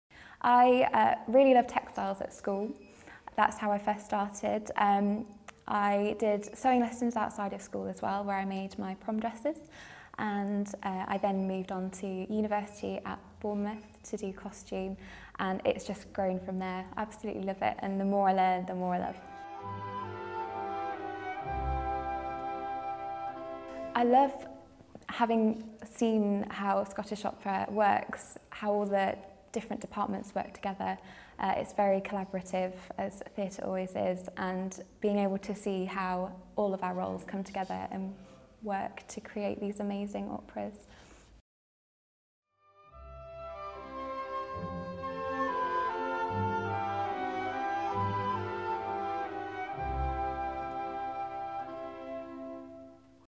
Scottish Opera Emerging Artist 19/20: Jasmine Clark
Meet Jasmine Clark, our 19/20 Emerging Artist costume trainee